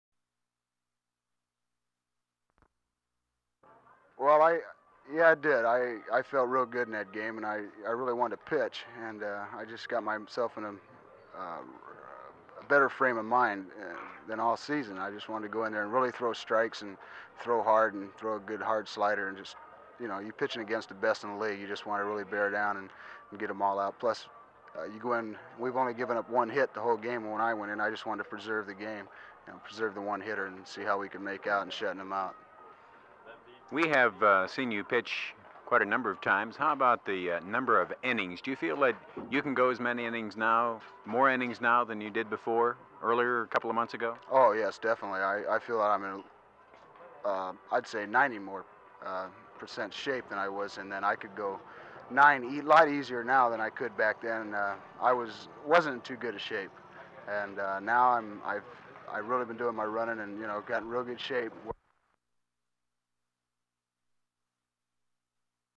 07/25/68 Louis Stephen Marone, pitcher for the Salem, Va., Rebels baseball team, divulges why h...
3139
LIVE
 SALEM-MANAGER...DON HOAK...HAS USED, LOU MARONE---AS, BOTH, A STARTER...AND A RELIEVER.
MARONE PITCHED, 3-INNINGS...OF HITLESS-BALL...IN THE ALL-STAR GAME...LAST-WEEK. AND, APPEARS, TO BE...MUCH-STRONGER, NOW.
 WE ASKED, MARONE---IF THE ALL-STAR GAME...WAS A BIG, TURNING-POINT...FOR HIM. (X)
COLOR-FILM SOUND LOU MARONE (:55) (OPEN:"WELL, I FELT. ")
 7/25/68 BW
 CLOSE:"...IM IN REAL GOOD SHAPE.") (X)
LIVE
IN ADDITION...TO THE SALEM-AT-WILSON GAME, TONIGHT THE "CAROLINA-LEAGUE" SCHEDULE...HAS. LYNCHBURG, AT PORTSMOUTH. RALEIGH-DURHAM, AT PENINSULA. WINSTON-SALEM, AT KINSTON. BURLINGTON, AT GREENSBORO. AND, ROCKY-MOUNT, AT HIGH-POINT==THOMASVILLE.
LIVE